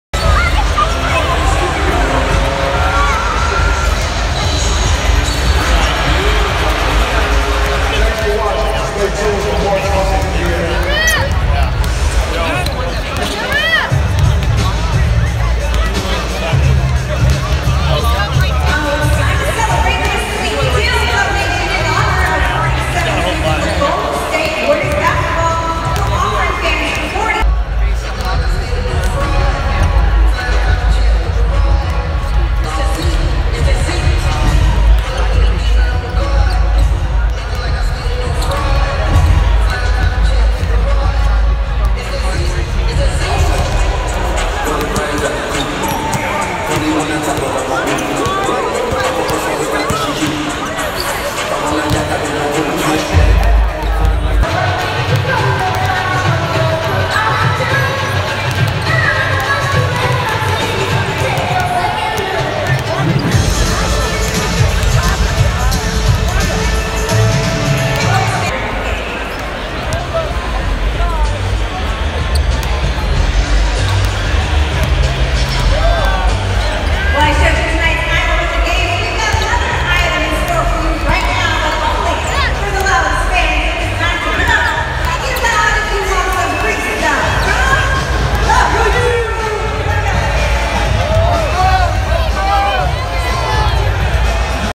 Pregame KD (Kevin Durant) dunk and Klay splashes, plus Draymond, before Phoenix Suns at Oracle Arena
From pregame before Warriors (2-1) vs Phoenix Suns at Oracle Arena.

NOTE: Views from courtside that are not interviews are, per NBA rules, limited to a maximum of two minutes. Also, we did not modify the video to account for stability due to timeliness of upload (the sooner the upload for you, the better, is the mantra here).